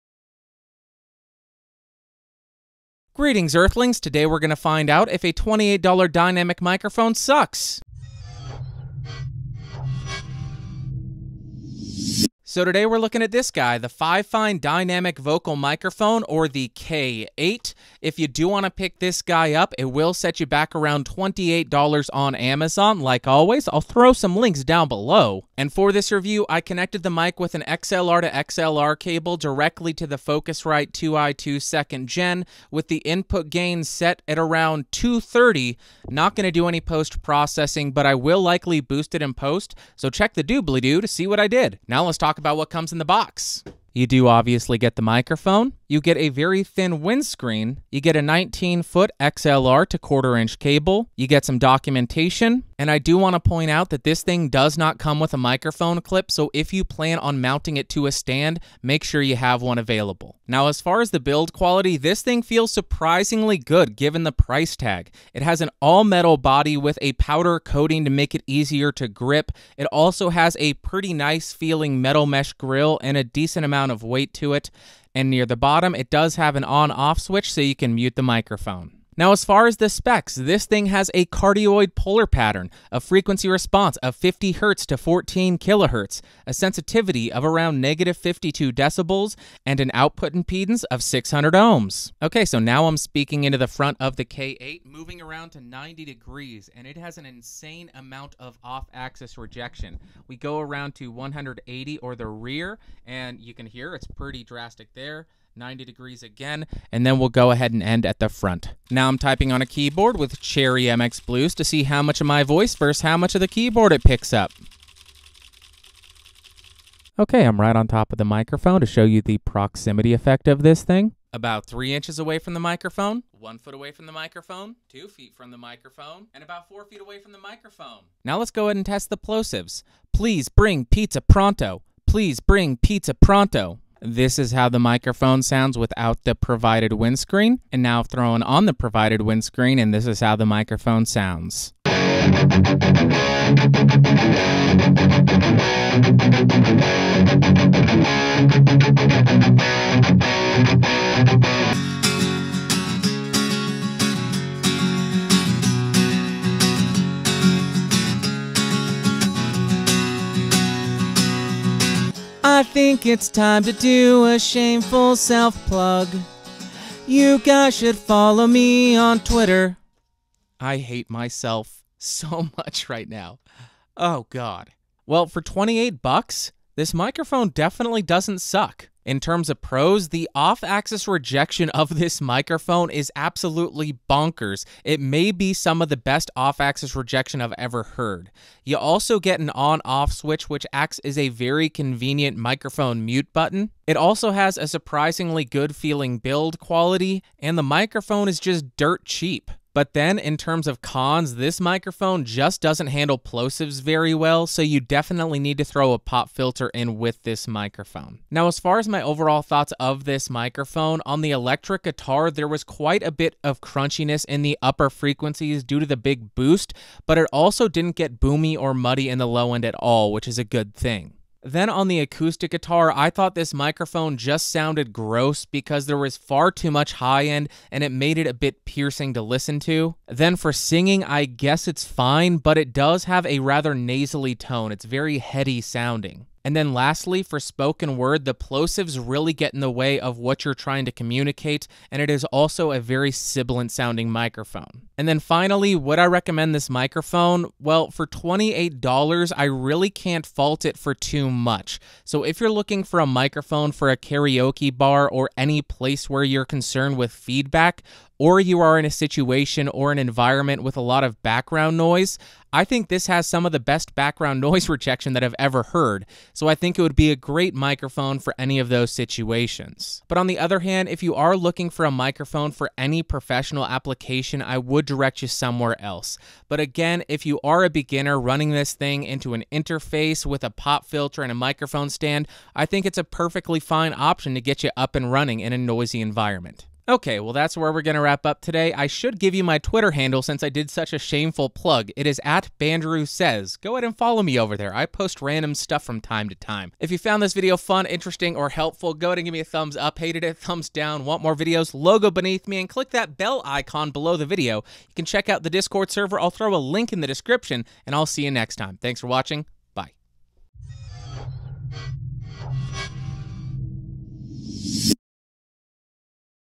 Fifine Dynamic Vocal Microphone Review / Test
Today we’re looking at a cheap budget dynamic microphone, the FiFine K8, or as they call it the "FiFine Dynamic Vocal Microphone Cardioid Handheld Microphone On/Off Switch Karaoke, Live Vocal, Speech etc.”. This microphone is rather boosted in the higher frequencies, leading it to sound sibilant, and it also suffers from plosives. However, it does have one of the best off axis rejections I’ve encountered on a mic. So if you’re in a noisy environment, this microphone may be a good option for you.

00:00 - Intro / Price
00:25 - Disclaimers / Setup
00:41 - What’s in the Box
01:00 - Build Quality
01:19 - Specifications
01:32 - Polar Pattern Test
01:48 - Background Noise Test
01:57 - Distance Test (Including Proximity)
02:08 - Plosive Test
02:14 - Windscreen Test
02:20 - Music Test
02:54 - Pros & Cons
03:28  - Overall thoughts
04:04 - Conclusion / Recommendation
04:46 - Outro